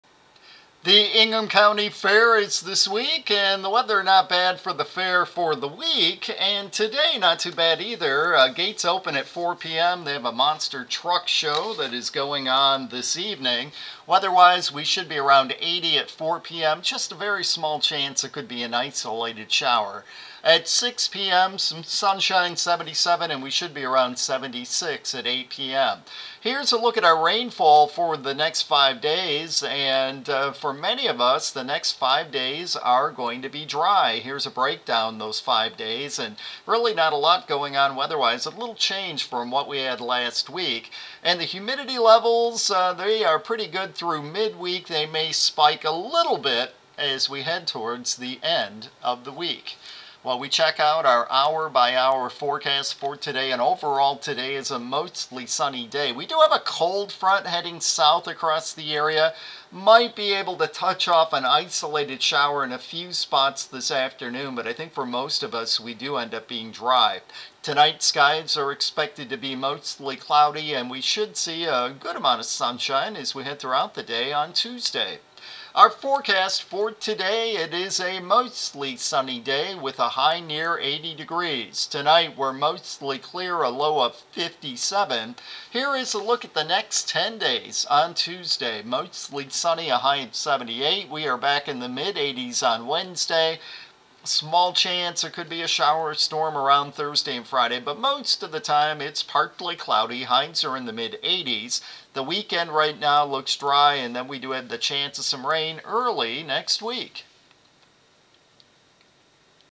Mainly Dry Week Ahead
First Alert Weather Monday morning webcast from WILX News 10